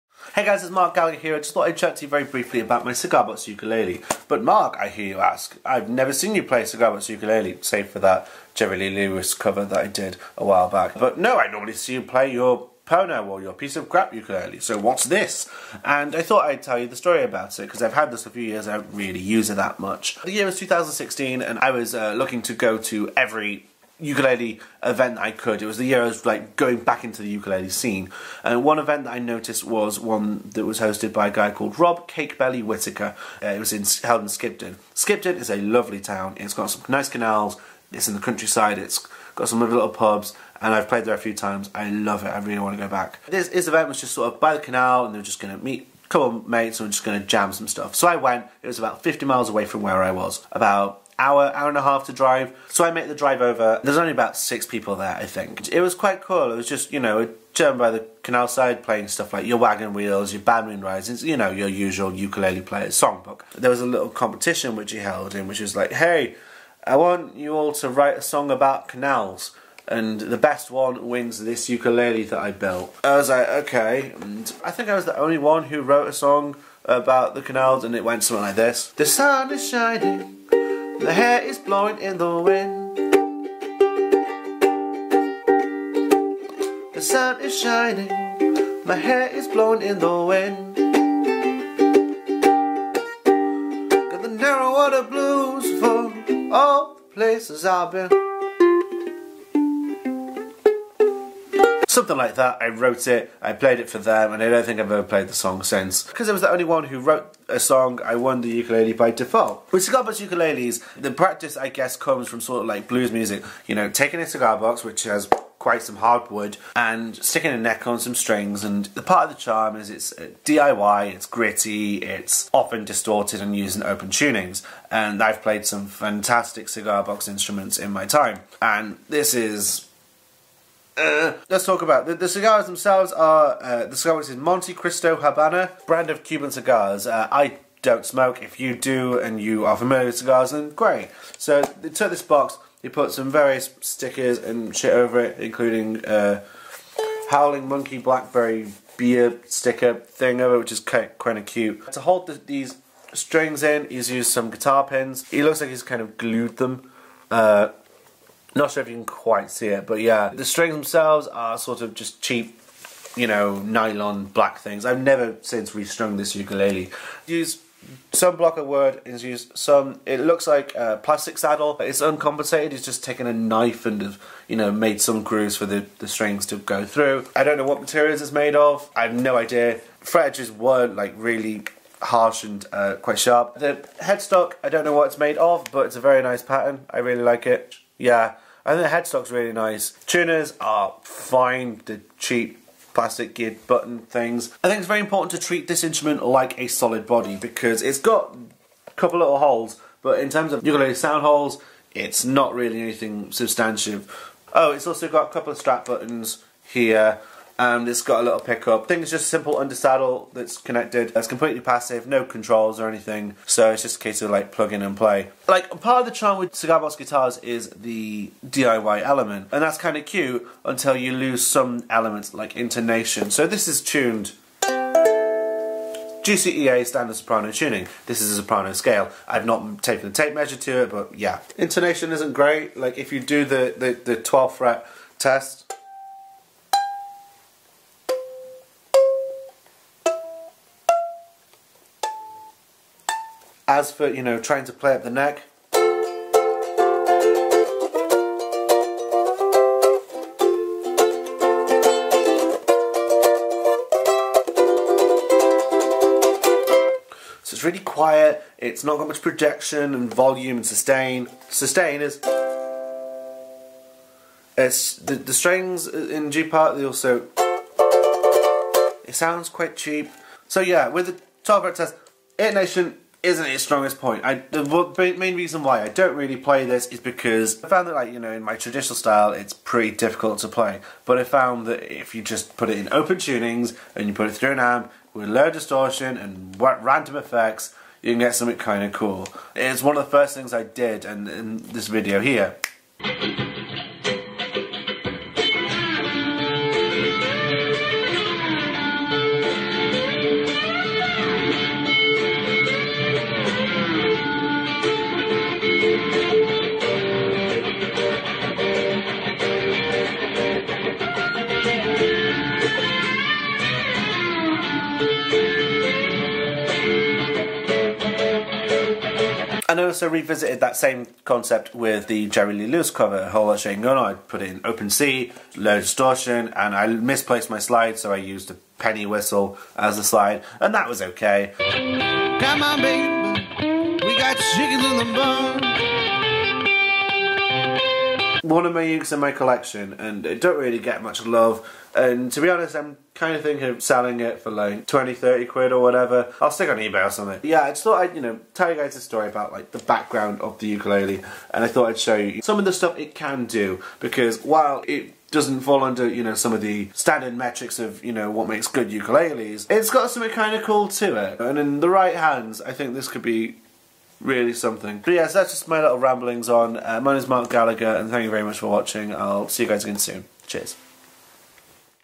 A Look At My Cigar Box Ukulele
Last week you saw me do a mental Jerry Lee Lewis cover with a Cigar Box Uke and a tin whistle. You probably didn't know I even had a cigar box uke, and I thought I'd tell you the story behind it and have a little look at this little instrument.